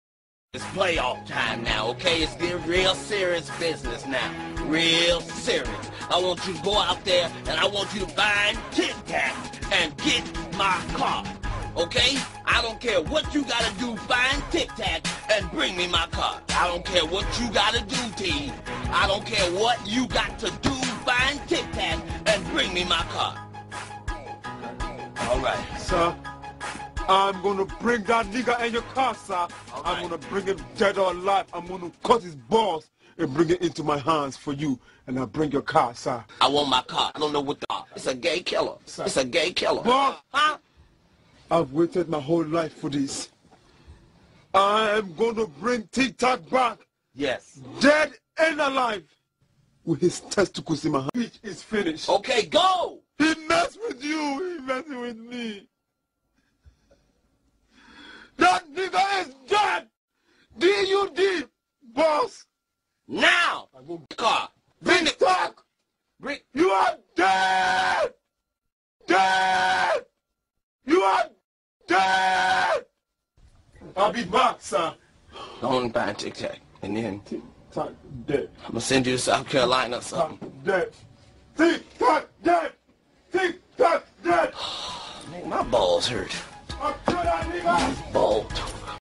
Katt Williams orders African Gangster,Master P to get his car....Repo sketch
Ganster desperately trying to impress Boss(katt williams)